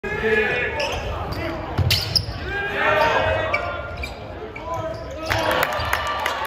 5647F277 9A7E 4F92 9844 9161E37877D4
Walker Johnson hitting a 3